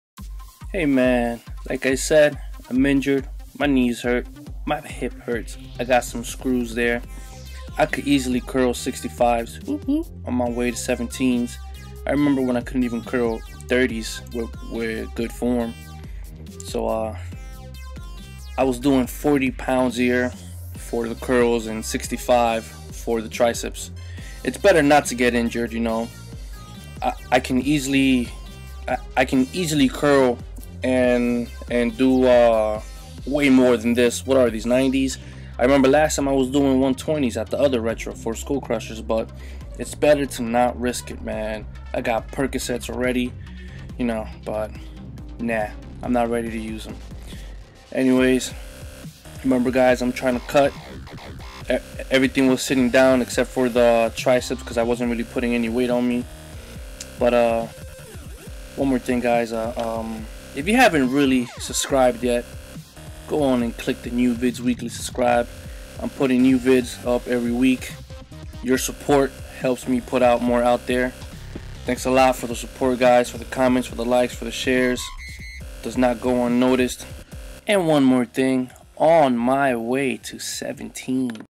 Post Surgery Arm Workout
So am still trying to curt here, doctors appointment tomorrow ...i think tomorrow they will let me kno if i can go back to work, but the way its looking right now my left knee won't let me do shit meng